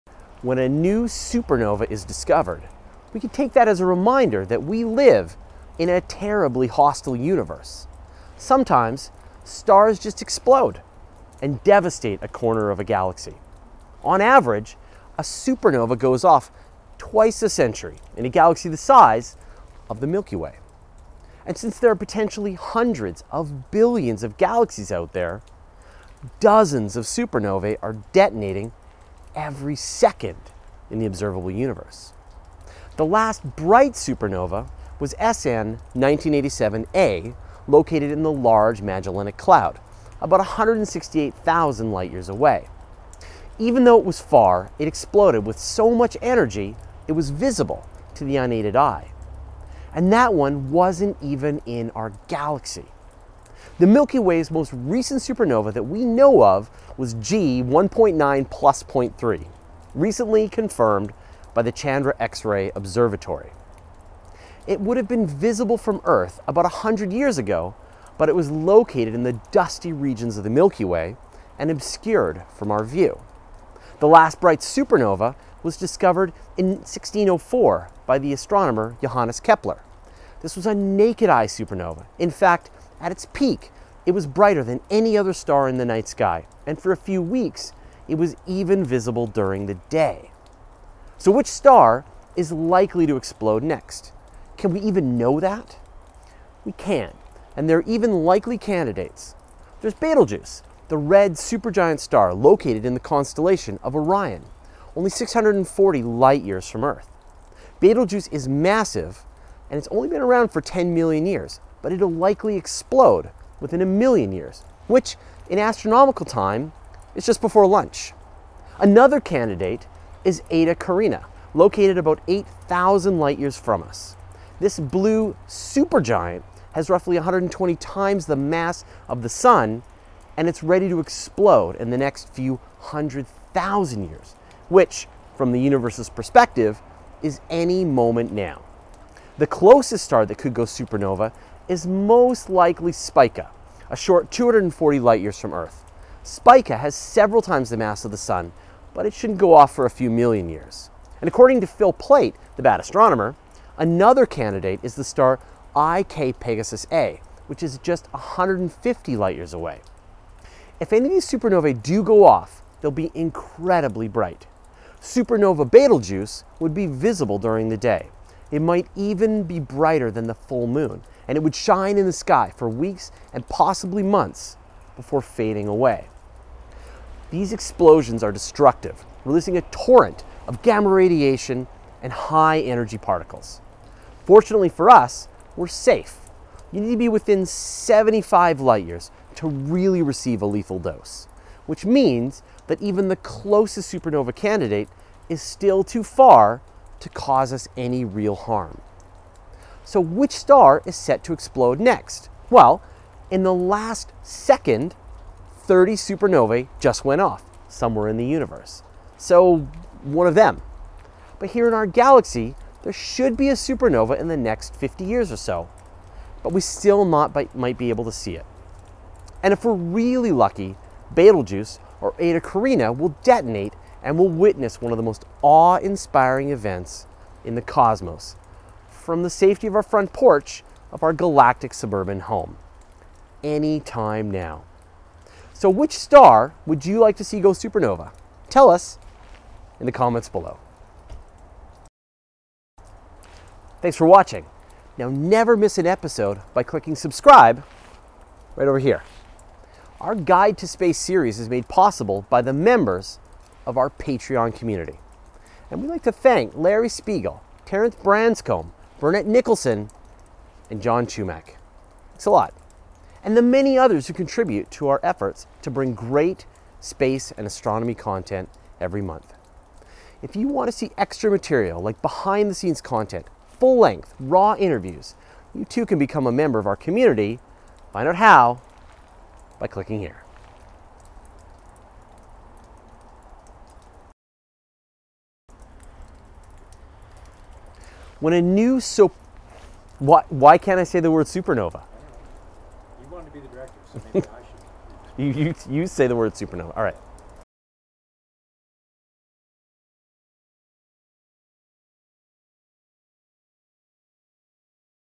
Which Star Will Explode Next? Supernova Precursors in Our Backyard
Come on Betelguese, explode already. Or maybe it'll be Eta Carinae. Which of the billions of stars in the galaxy can we count on to explode next, and when?